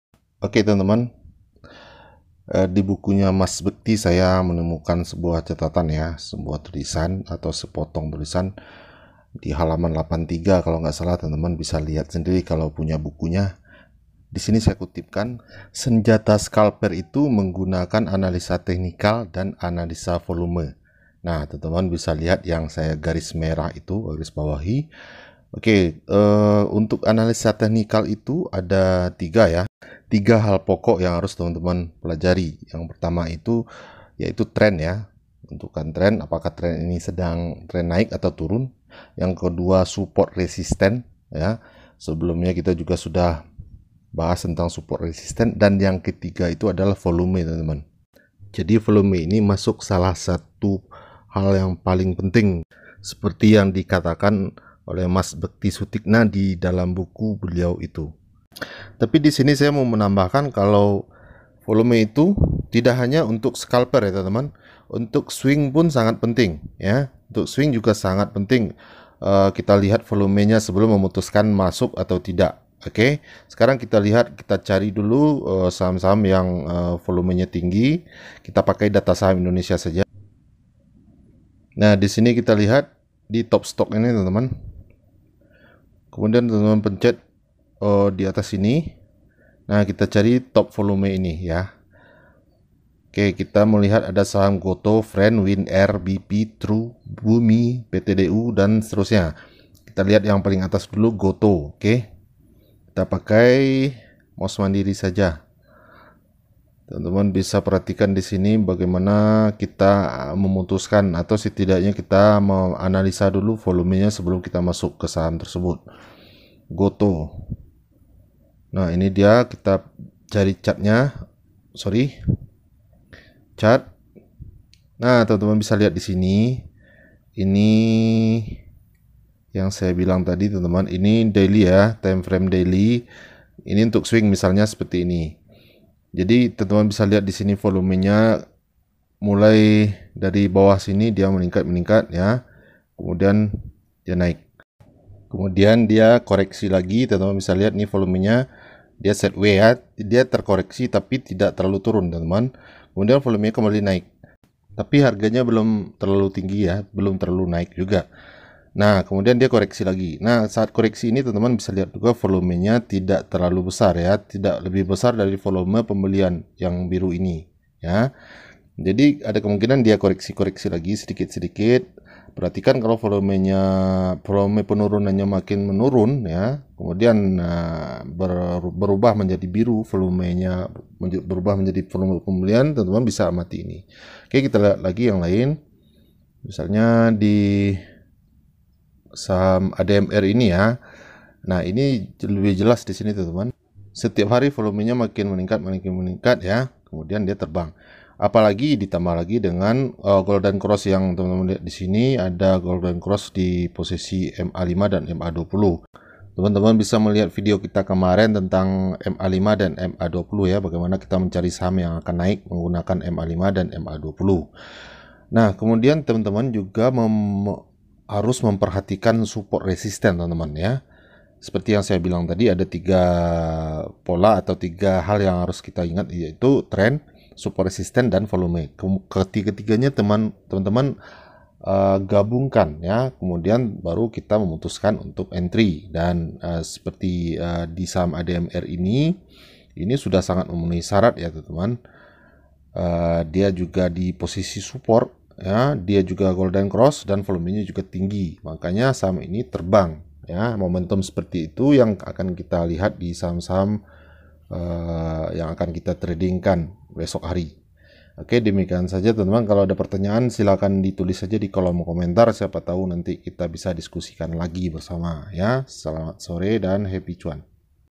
Volume Saham Menurut Mas Bekti Sutikna
Cara menemukan saham yang akan naik dengam melihat volume, di terjemakan dari buku The Super Scalper Bekti Sutikna